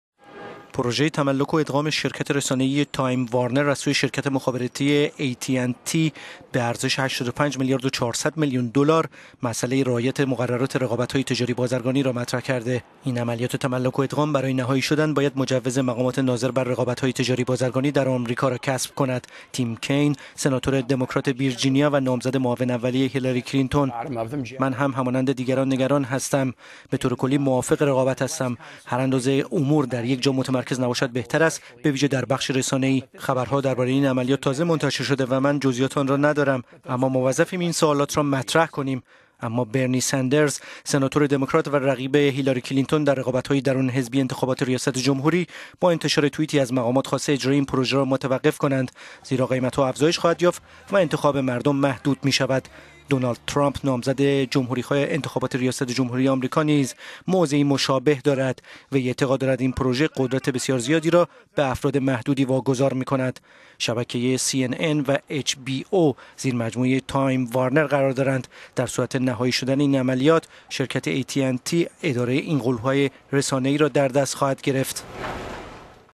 دموکراتها و جمهوری خواهان، مخالف پروژه تملک و ادغام تایم وارنر - economy
پروژه تملک و ادغام شرکت رسانه ای تایم وارنر از سوی شرکت مخابراتی ای تی اند تی به ارزش ۸۵ میلیارد و ۴۰۰ میلیون دلار مسئله رعایت مقررات رقابتهای تجاری بازرگانی را مطرح کرده است.

عملیات تملک و ادغام برای نهایی شدن باید مجوز مقامات ناظر بر رقابتهای تجاری بازرگانی در ایالات متحده را کسب کند.

تیم کین، سناتور دموکرات ویرجینیا و نامزد معاون اولی هیلاری کلینتون دراینباره گفت: «من هم همانند دیگران نگران هستم و سئوالاتی را مطرح می کنم. بطور کلی موافق رقابت هستم. هر اندازه امور در یک جا متمرکز نباشد بهتر …